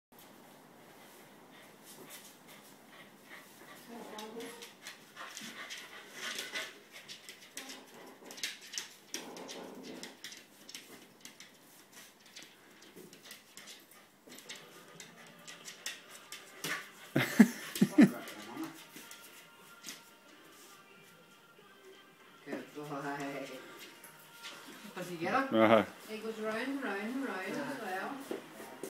Shihtzu Begs For Dentix!
Me Mums cute Shihtzu begging for food...cool or what?